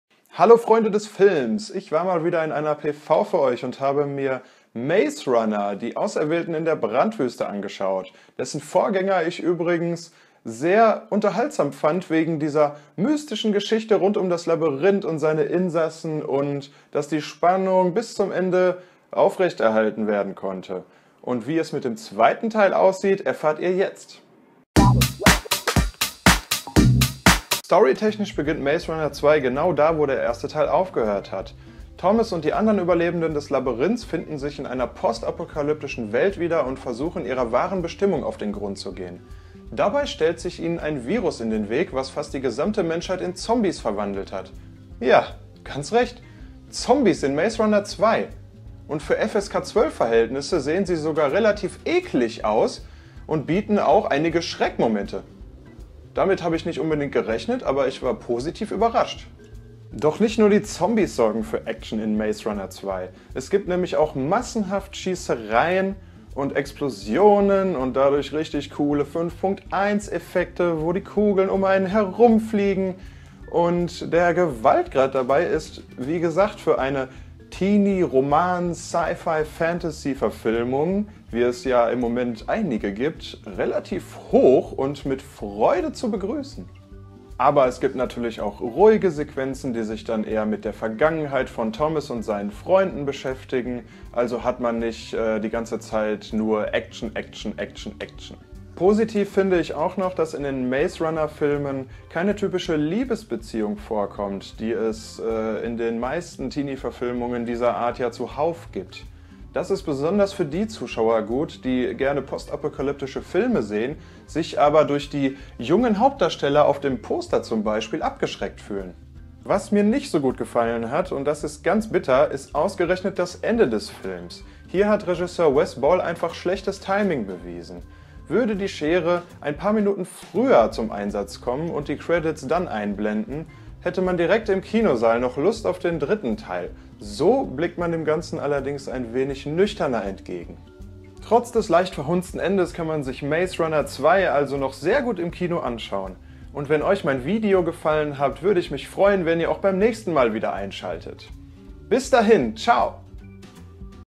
MAZE RUNNER 2 - DIE AUSERWÄHLTEN IN DER BRANDWÜSTE | KRITIK (deutsch)
Maze Runner - Die Auserwählten in der Brandwüste (2015) | Kinostart: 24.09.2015 | Lauflänge: 133 Min. | FSK: 12 | Verleih: 20th Century Fox

Kurzinhalt:
Nach dem Entkommen aus dem Labyrinth sind Thomas (Dylan O’Brien) und seine Mitstreiter noch immer nicht in Sicherheit. Stattdessen wartet eine neue  Herausforderung auf sie: Betrogen von der geheimnisvollen Organisation WCKD landen sie in der Brandwüste, einer trostlosen Landschaft, in der sie sich erneut unglaublichen Gefahren stellen müssen. Die Gruppe um Thomas verbündet sich mit Widerstandskämpfern, auf die sie treffen, um die schockierenden Pläne der mächtigen Institution WCKD zu enthüllen und das Rätsel, um die Brandwüste und warum sie hier sind zu lösen…